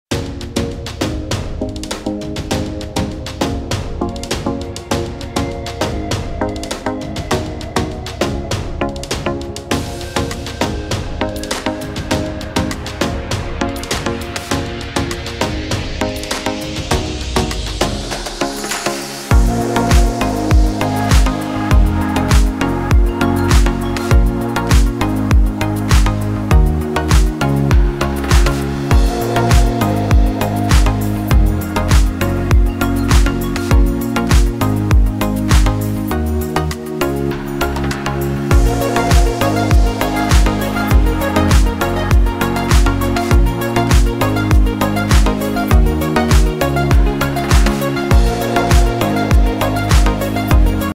4K - Albufeira beach in Algarve - Portugal - aerial drone video - Summer 2022
Short overview on Albufeira beach , one of the best locations of the amazing Algarve in Portigal.